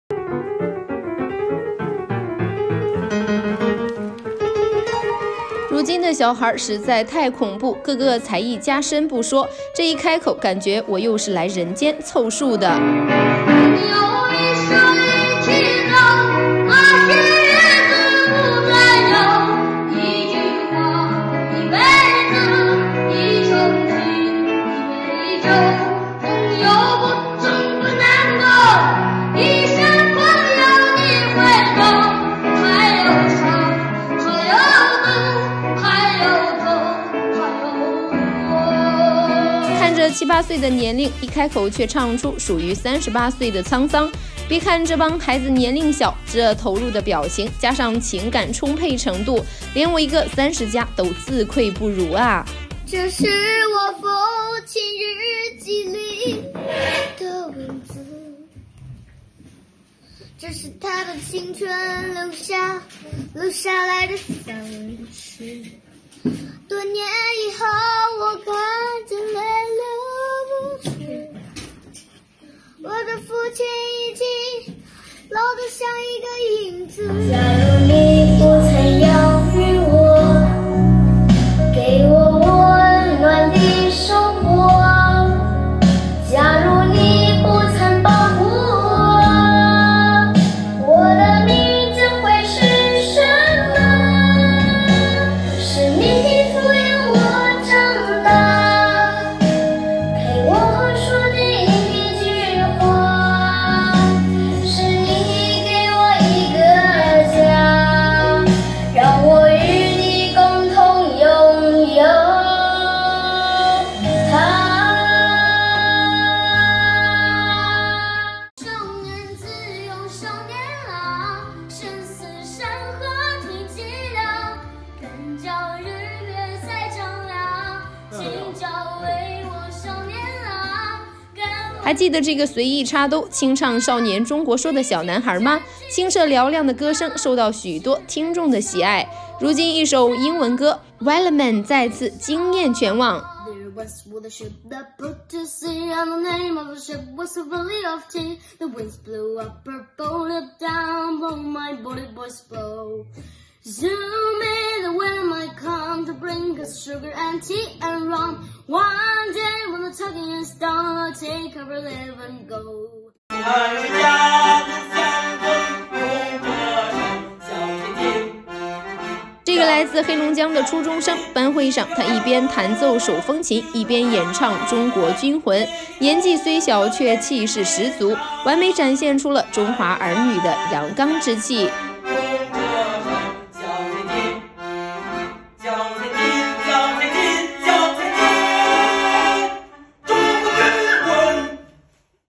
“满级小孩”开口王炸？8岁唱出38岁的沧桑，气质拿捏死死的！
❤ 每天都有新视频！！今天开始报名！！